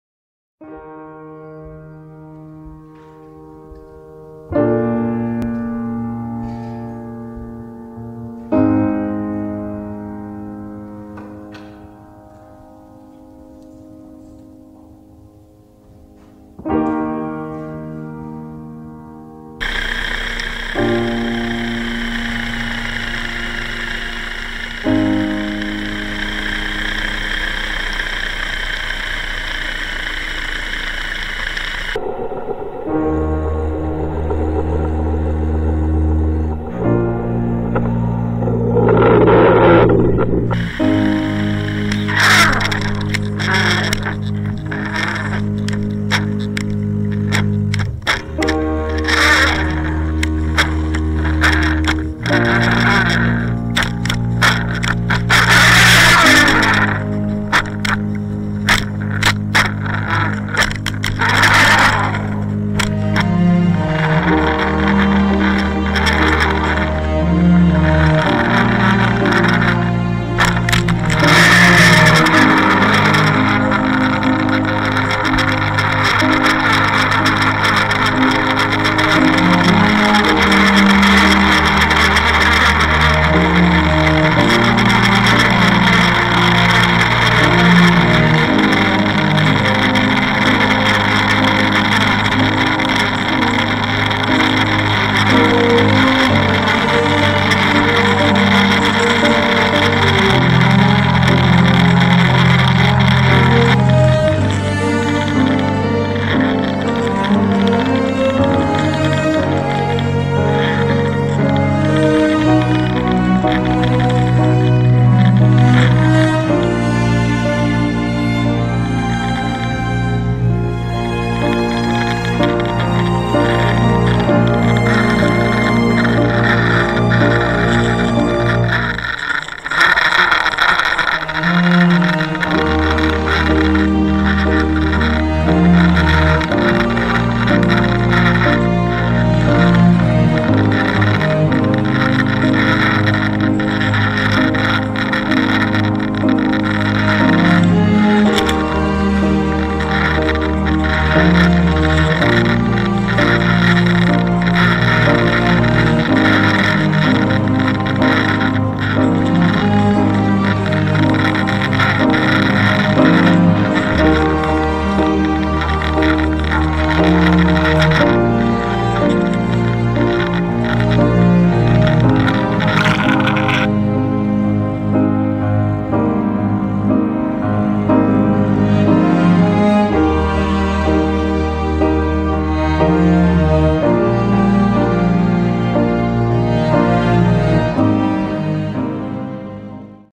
Albacore underwater strike
Seaforce One Fishing team Luciana and Alfonso filmed underwater albacore strike in  Gallura clear water of Sardinia North East